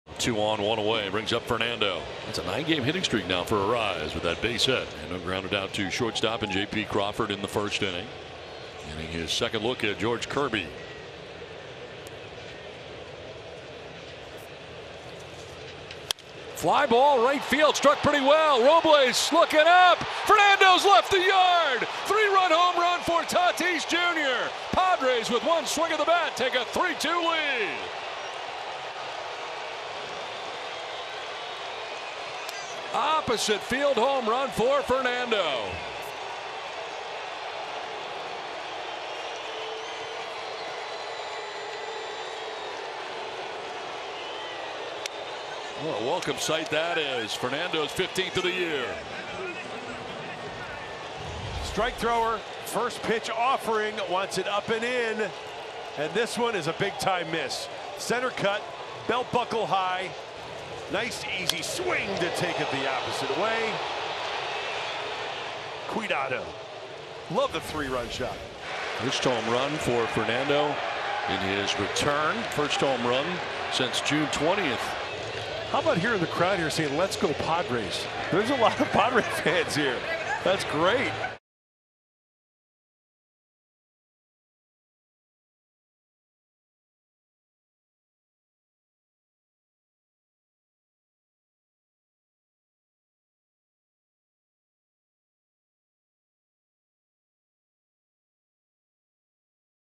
Fernando Tatis Jr.'s first home run since returning from the IL puts the Padres ahead!
Fernando Tatis Jr. slugs a 3-run homer for the Padres!